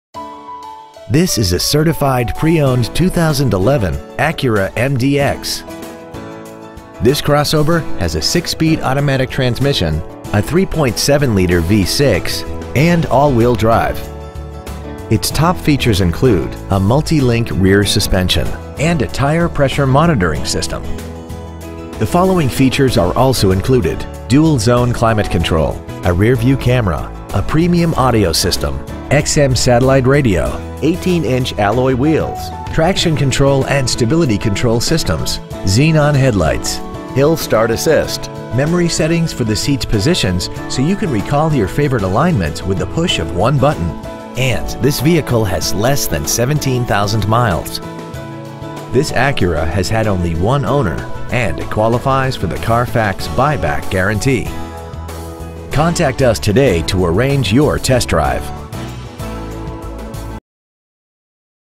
Pre-Owned 2011 Acura MDX Canton CT 06019
We have been honored to serve the Canton CT area , we promise that your experience at our dealership will exceed your expectations !

 Year : 2011
 Make : Acura
 Model : MDX
 Engine : Gas V6 3.7L/224
 Trans . : Automatic
 Exterior : Dark Cherry Pearl
 Miles : 16,325
 Interior : Parchment

 75 Albany Turnpike
 Canton , CT 6019